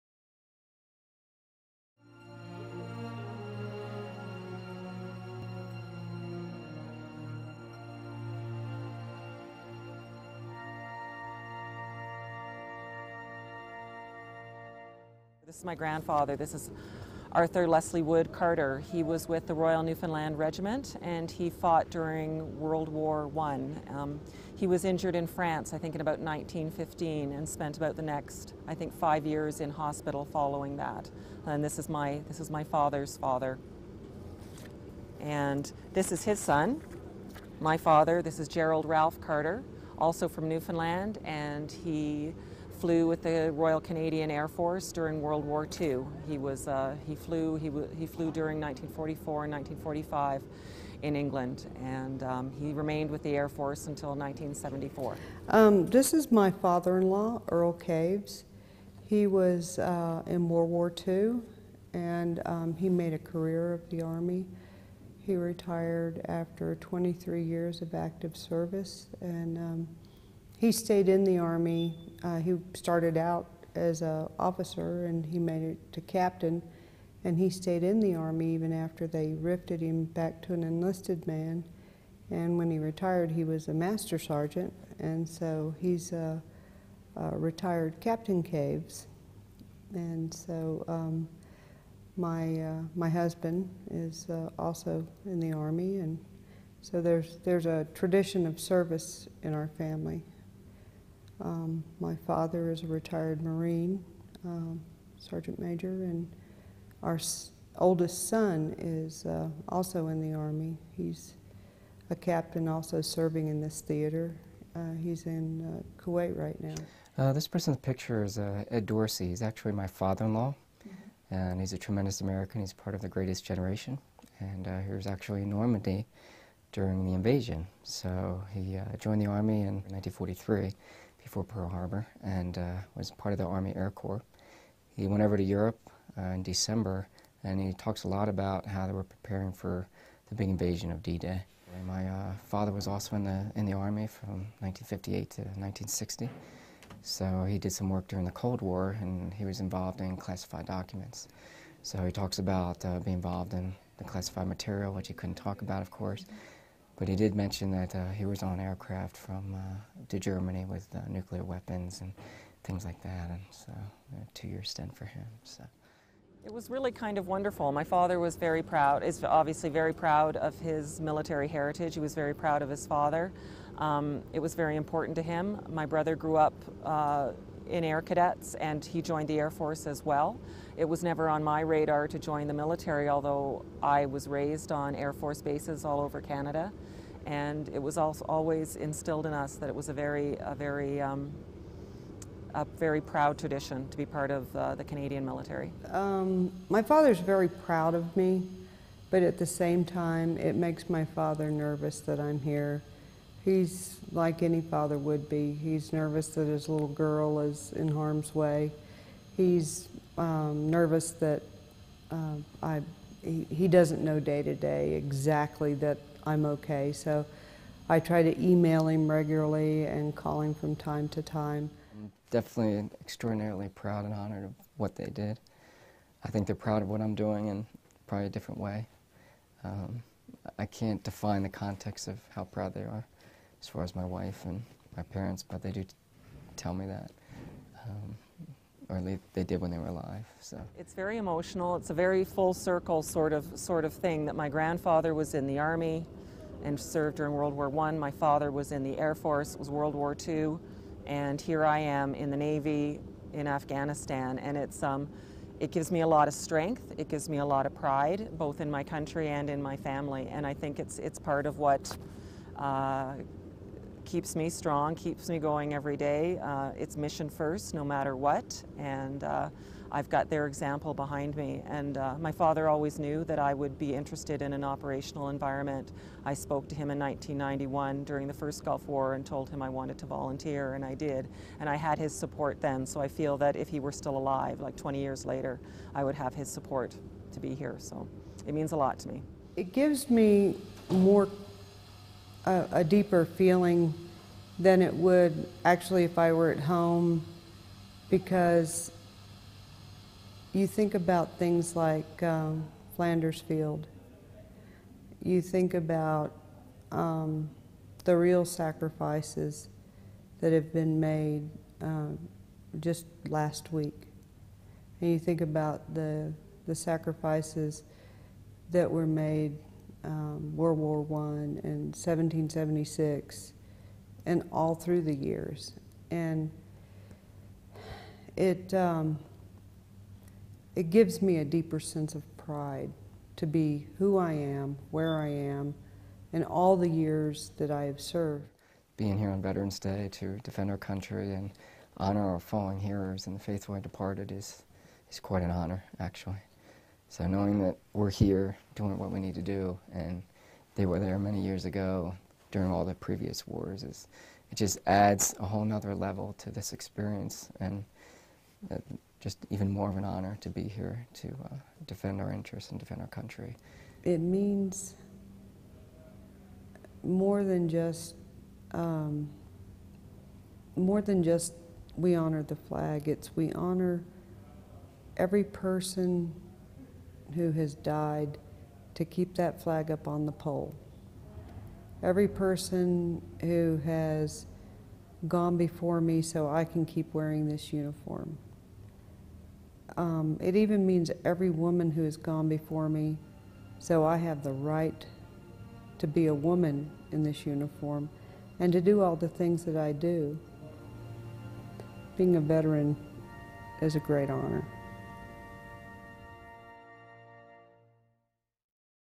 VETERANS DAY -Military personnel in Afghanistan talk about their service (Episode 3)
Video production of 3 service members (U.S. and Canadian) speaking about members of their families who are Veterans and how that has influenced their lives, especially while serving in Afghanistan on this day. Canadian Lieutenant JoAnne Carter - Advisor, Afghan National Civil Order Police, U.S. Army Lieutenant Col. Cheryl Caves - Force Developer, Police Force Integration and U.S. Navy Commander Brendan Melody, Chief of Staff - Commander Surgeon Off. Produced by SSgt. Holly Roberts. Also available in High Definition.